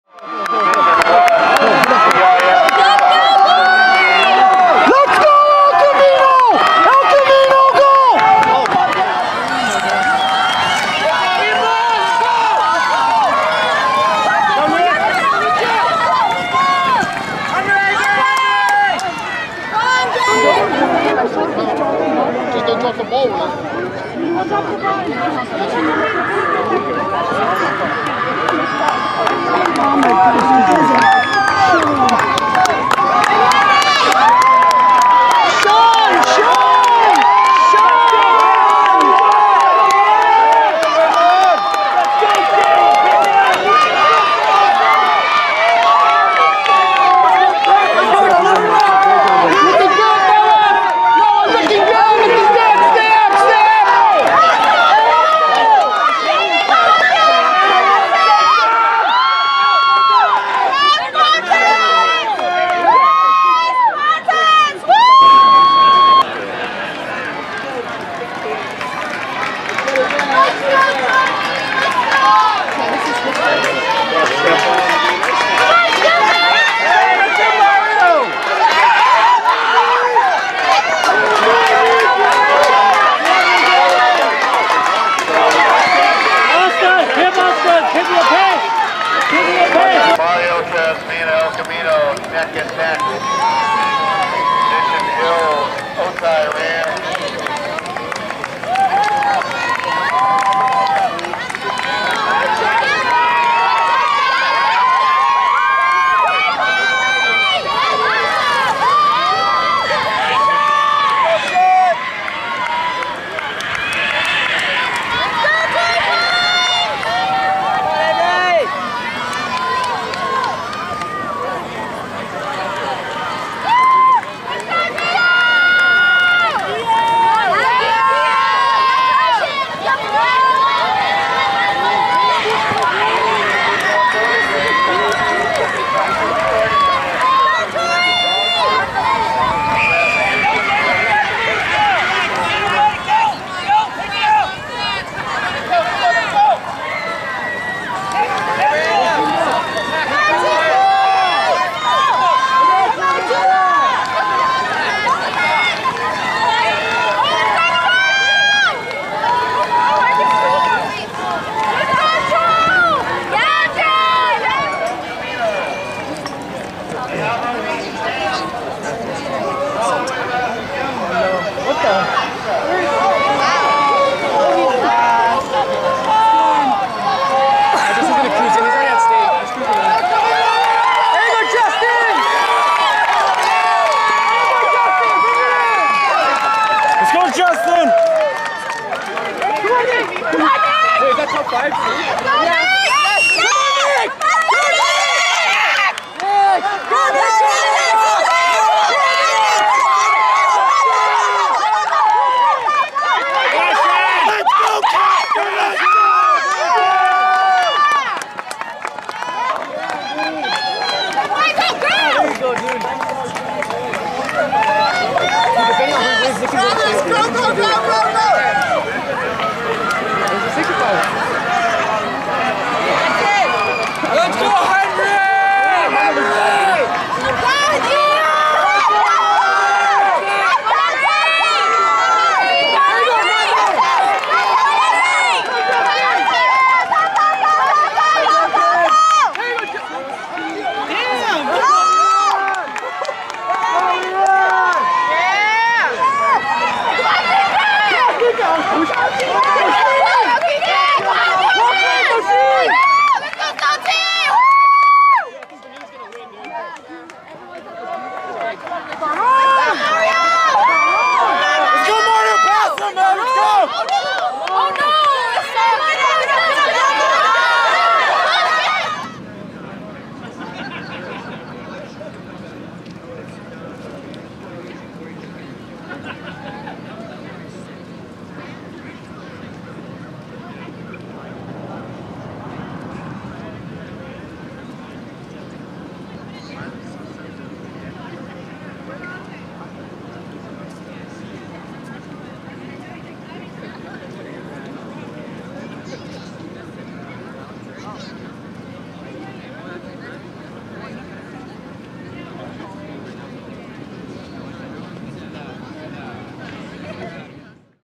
17' CIF-SD XC Finals Boys D1
2017 CIF San Diego Section Cross Country Finals
Boys Division 1
Morley Field 11/18/17

Top 10:
1. 12 Mario Trasvina 15:23.7 El Camino (Oceanside)
2. 12 Stefen Rasmuson 15:25.0 Mira Mesa
3. 11 Justin Mulvany 15:27.3 Otay Ranch
4. 12 Isaiah Labra 15:41.8 Bonita Vista
5. 12 Nicholas Albro 15:45.1 Olympian
6. 11 Sean Caster 15:45.9 Mission Hills
7. 11 Aidan Katson 15:45.9 San Marcos
8. 11 Jonathan Velasco 15:47.6 Mission Hills
9. 10 Jovanny Garcia 15:47.7 Vista
10. 12 Robert Bartsch 15:50.8 Torrey Pines